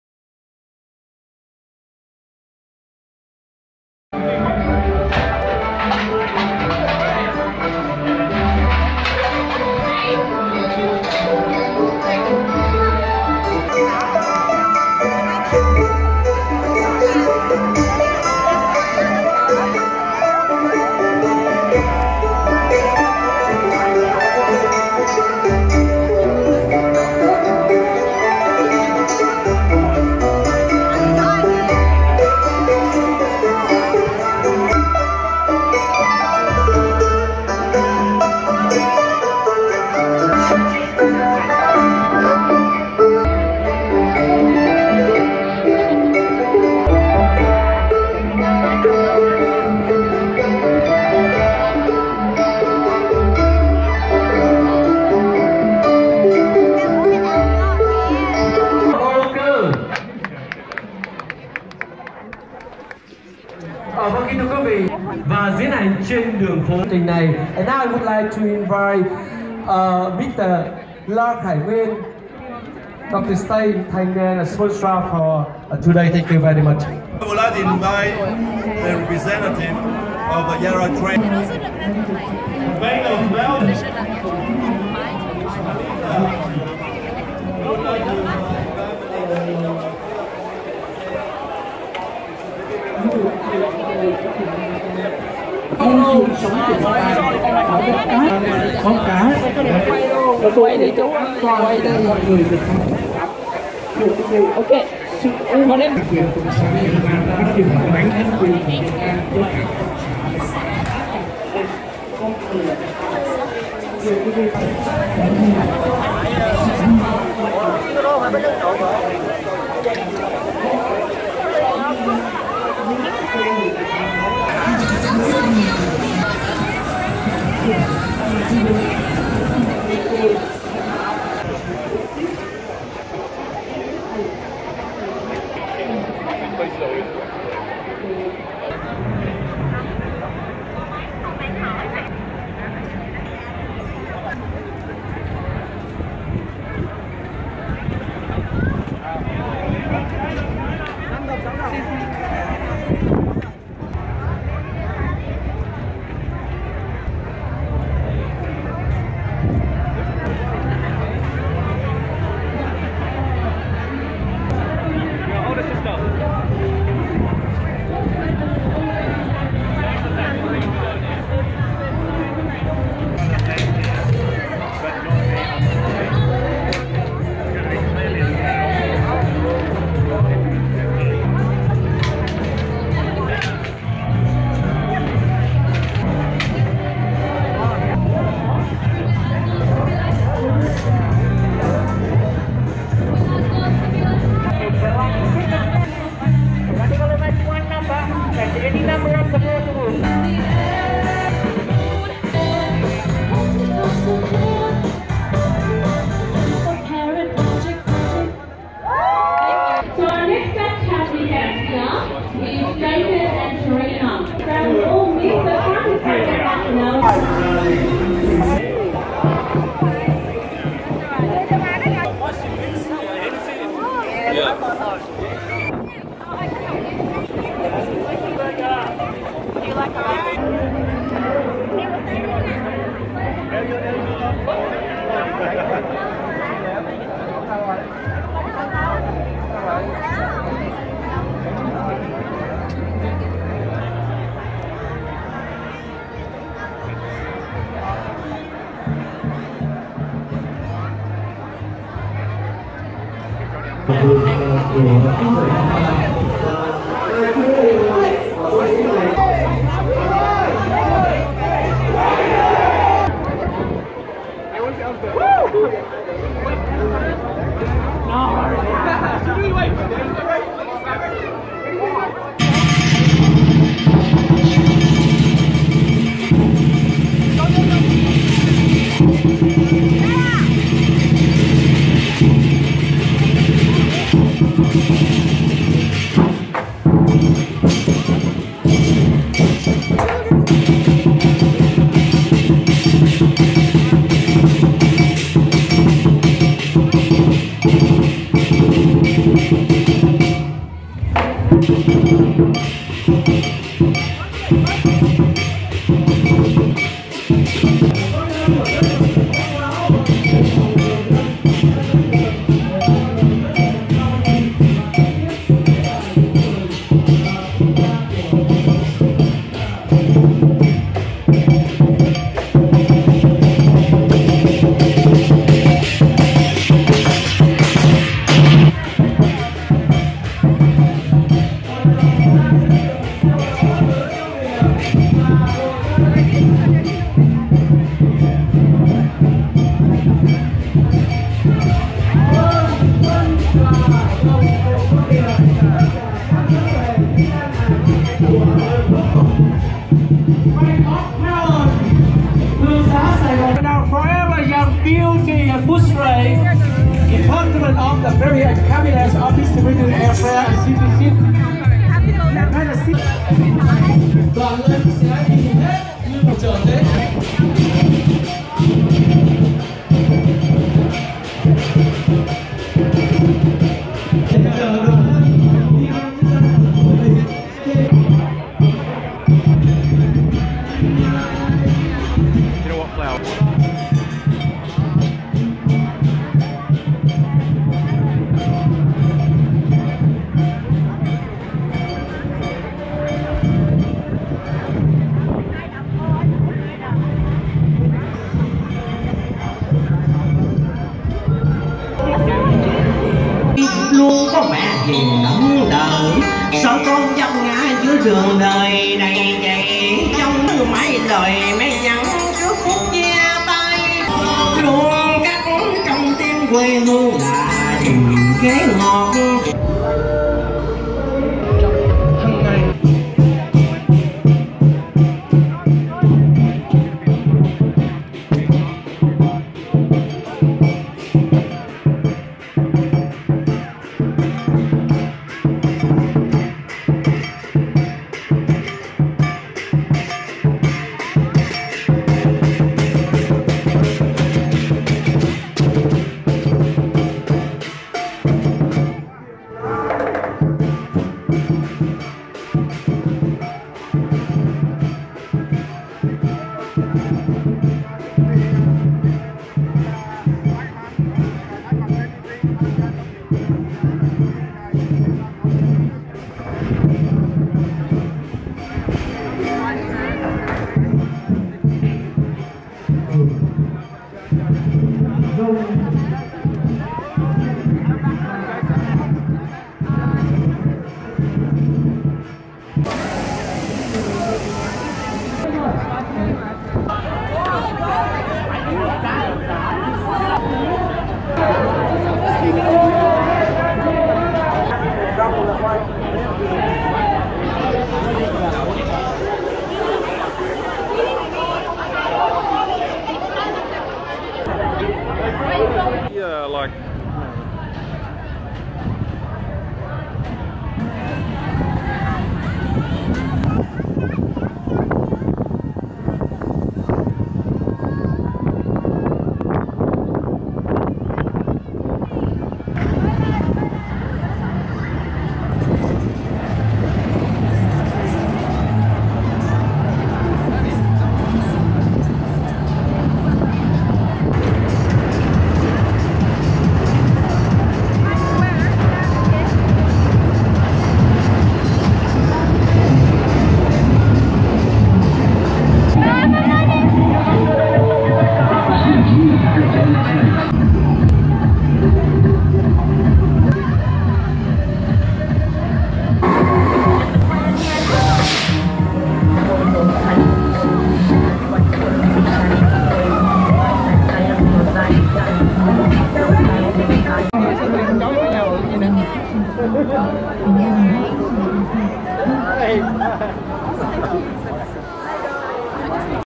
Lunar New Year Footscray
The 'East Meets West' Festival in Footscray to celebrate the Lunar New Year of the 'Year of the Fire Chicken'. Footscray is the main hub of the Vietnamese community in Melbourne.